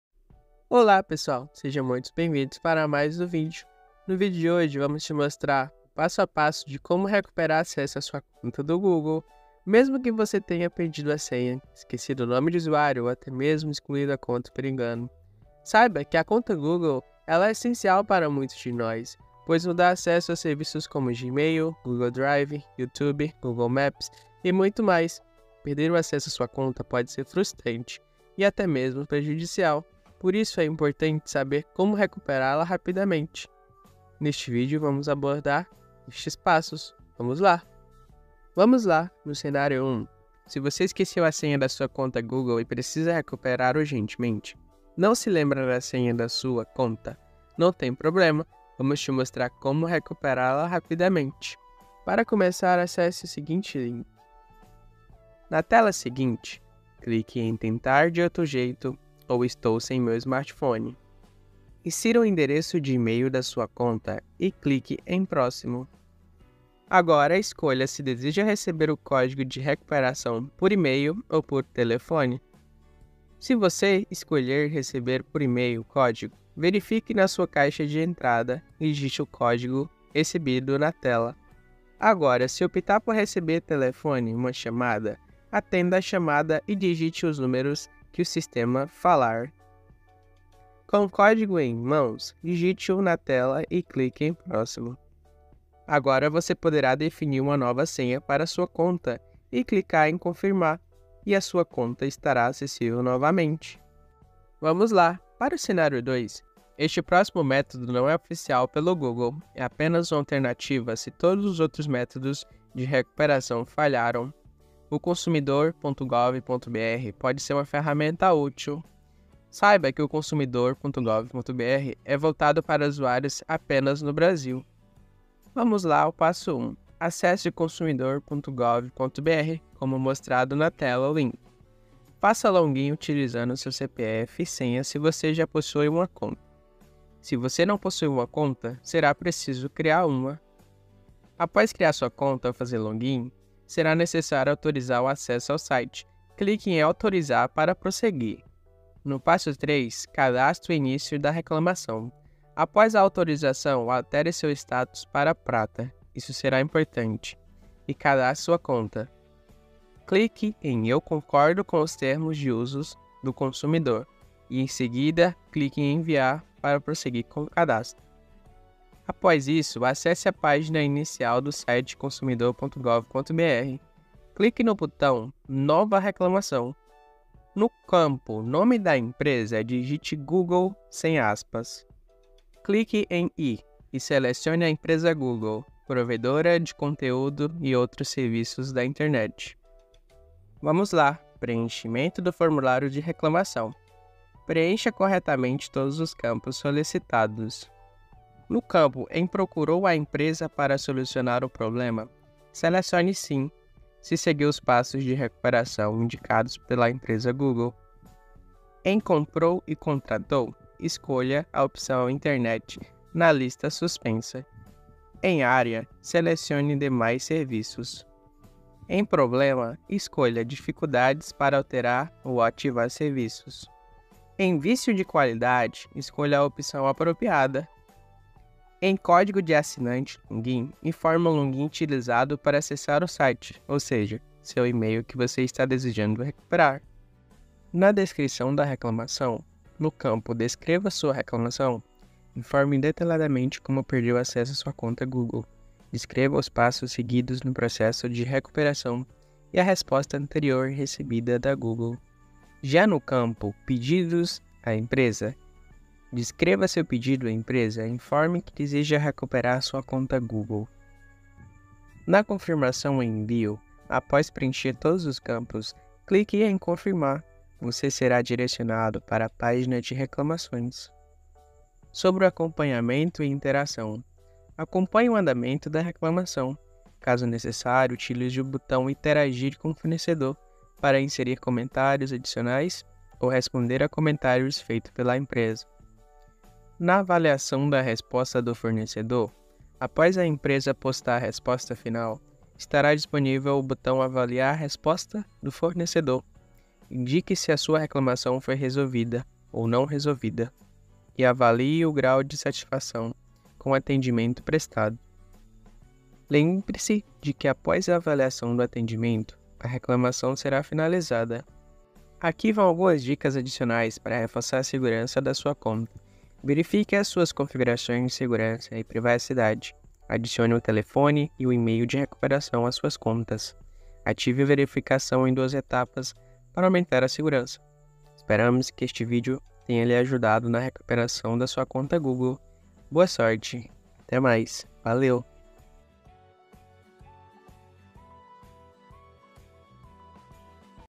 Como Recuperar CONTA GOOGLE em Diferentes cenários?
Neste vídeo Descubra como recuperar Conta Google Perdida? Recupere em Minutos!

Siga este guia passo a passo e recupere o acesso à sua conta, mesmo que tenha perdido a senha ou excluído por engano.

Dicas extras para sua segurança online.

Assista agora!